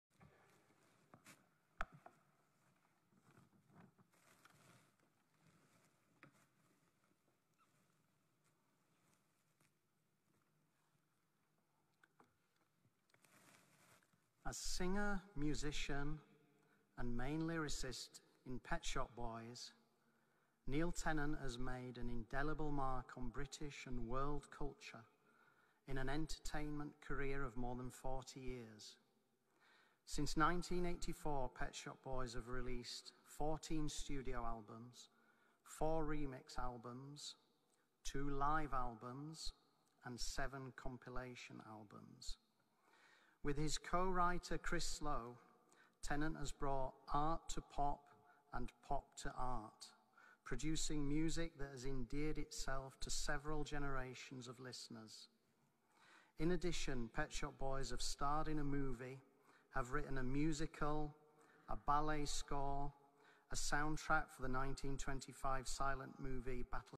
Neil Tennant - Honorary Doctor of Letters at Durham University
Neil Tennant made an Honorary Doctor of Letters by Durham University in a ceremony in Durham Cathedral (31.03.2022)